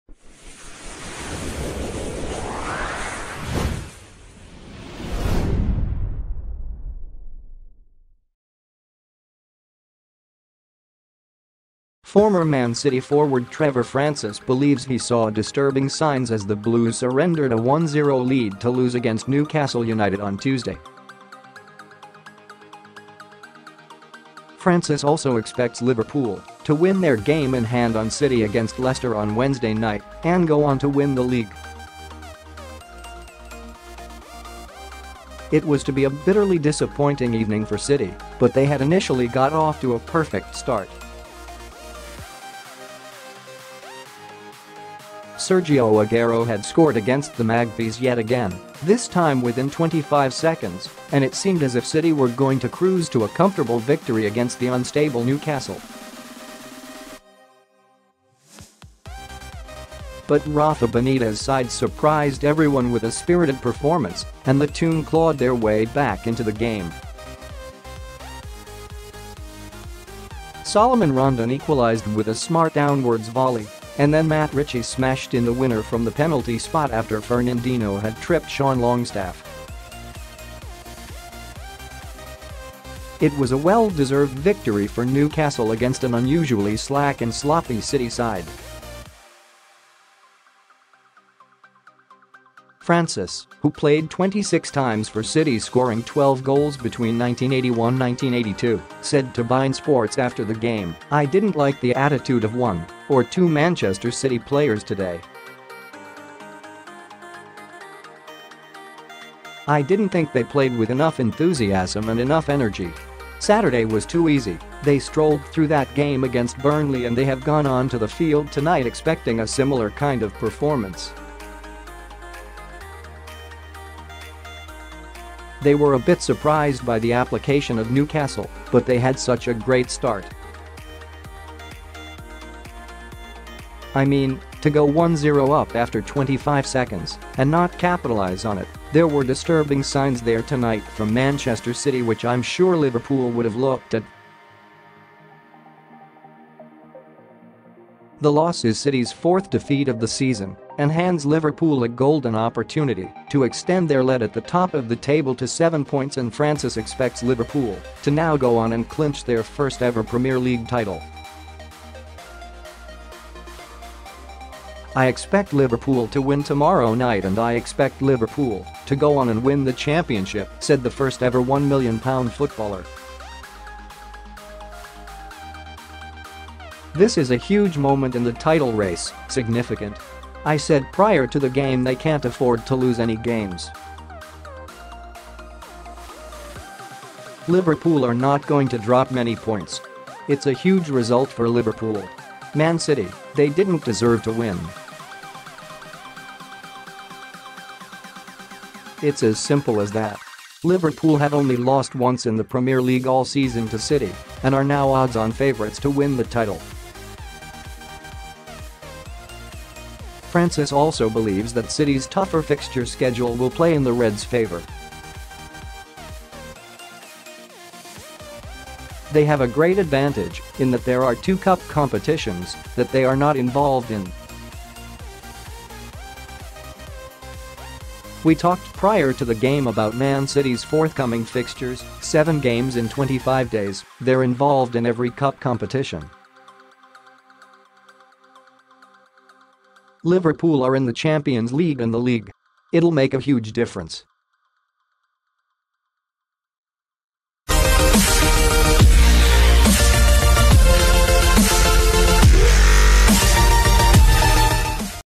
Ex-Man City striker saw 'disturbing signs' during Newcastle defeat
Former Man City forward Trevor Francis believes he saw 'disturbing signs' as the Blues surrendered a 1-0 lead to lose against Newcastle United on Tuesday.  Francis also expects Liverpool to win their game in hand on City against Leicester on Wednesday night and go on to win the league.   It was to be a bitterly disappointing evening for City , but they had initially got off to a perfect start.   Sergio Aguero had scored against the Magpies yet again, this time within 25 seconds, and it seemed as if City were going to cruise to a comfortable victory against the unstable Newcastle.  But Rafa Benitez' side surprised everyone with a spirited performance and the Toon clawed their way back into the game. Solomon Rondon equalised with a smart downwards volley and then Matt Ritchie smashed in the winner from the penalty spot after Fernandino had tripped Sean Longstaff.  It was a well deserved victory for Newcastle against an unusually slack and sloppy City side.   Francis, who played 26 times for City scoring 12 goals between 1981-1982, said to beIN Sports after the game: “I didn’t like the attitude of one or two Manchester City players today. I didn’t think they played with enough enthusiasm and enough energy.      “Saturday was too easy, they strolled through that game against Burnley and they have gone on to the field tonight expecting a similar kind of performance.  “They were a bit surprised by the application of Newcastle, but they had such a great start.  “I mean, to go 1-0 up after 25 seconds and not capitalise on it, there were disturbing signs there tonight from Manchester City which I’m sure Liverpool would have looked at.”    The loss is City's fourth defeat of the season and hands Liverpool a golden opportunity to extend their lead at the top of the table to seven points and Francis expects Liverpool to now go on and clinch their first ever Premier League title.  "I expect Liverpool to win tomorrow night and I expect Liverpool to go on and win the championship,"  said the first ever £1m footballer.  "This is a huge moment in the title race, significant. I said prior to the game they can't afford to lose any games. Liverpool are not going to drop many points. It's a huge result for Liverpool.  "Man City, they didn't deserve to win. It's as simple as that."     Liverpool have only lost once in the Premier League all season - to City - and are now odds on favourites to win the title.  Francis also believes that City's tougher fixture schedule will play in the Reds' favour.  "They have a great advantage in [that] there are two cup competitions that they are not involved in.  "We talked prior to the game about Man City's forthcoming fixtures, seven games in 25 days, they're involved in every cup competition. Liverpool are in the Champions League and the league. It'll make a huge difference."